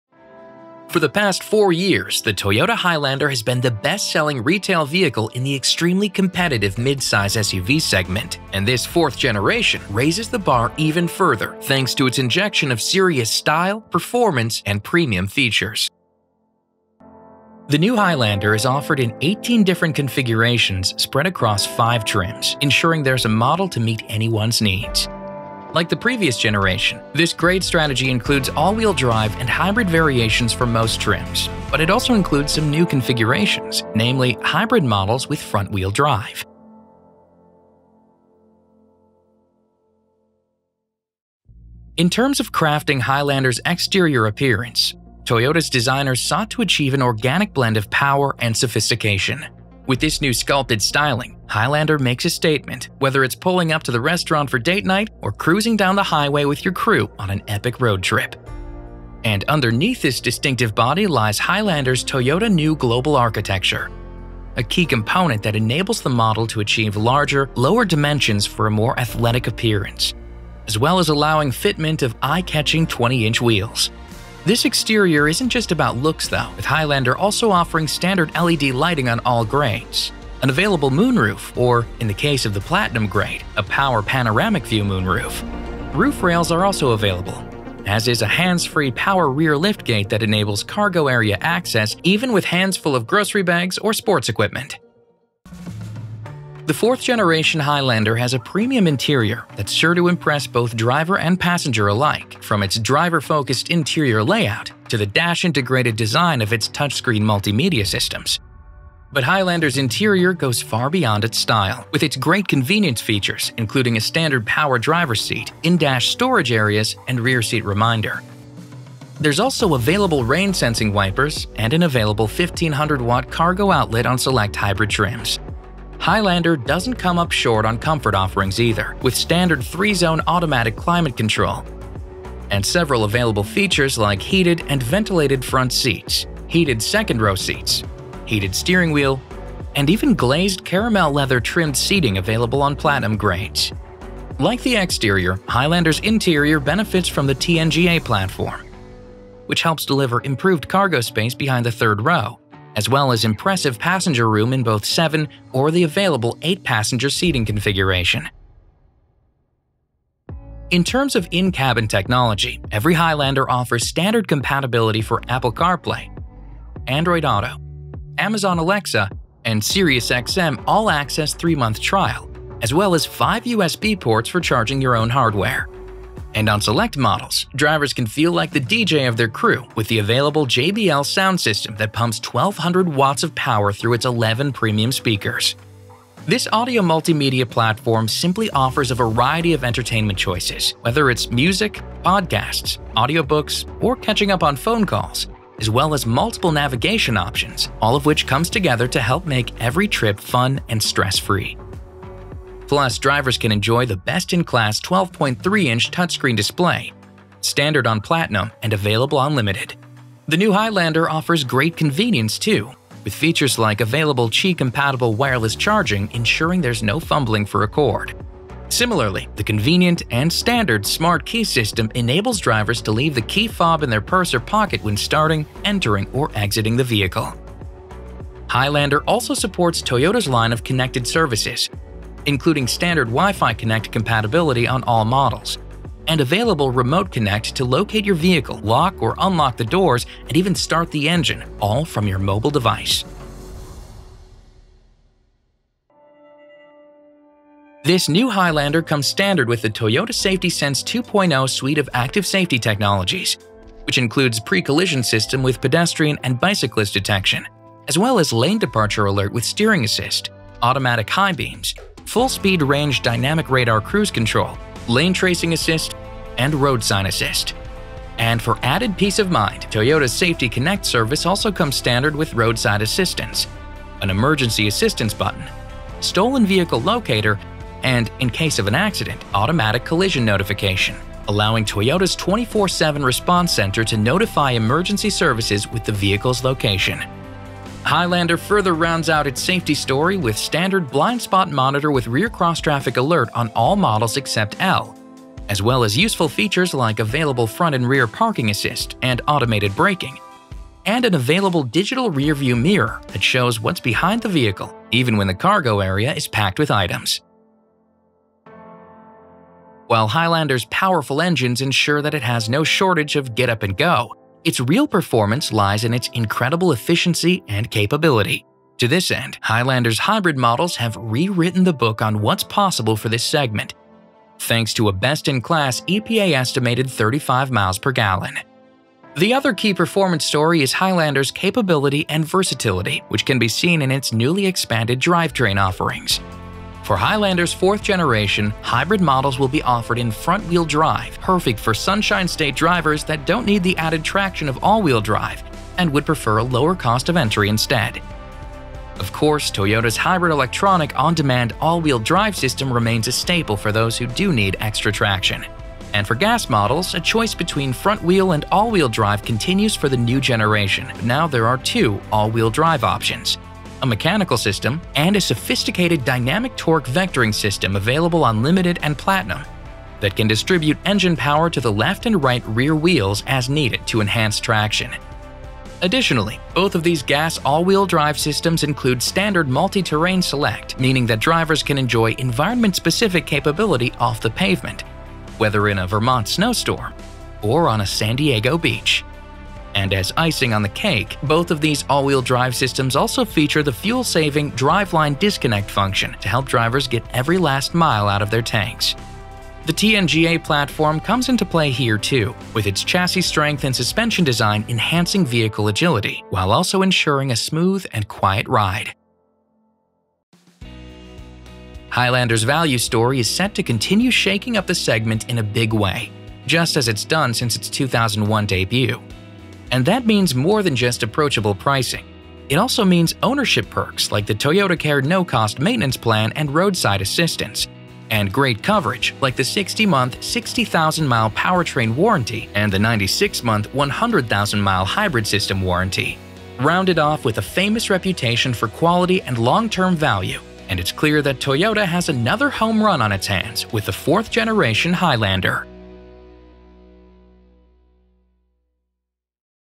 2020 Toyota Highlander Overview | Specs & Features
In this 2020 Highlander overview, learn about the specs and features in the newest 4th generation Toyota Highlander! Thanks to its premium style, performance, and premium features, learn why the new Highlander is on top of its game.

In this 2020 Toyota Highlander overview, learn about the new 4th generation Highlander's impressive specs and features!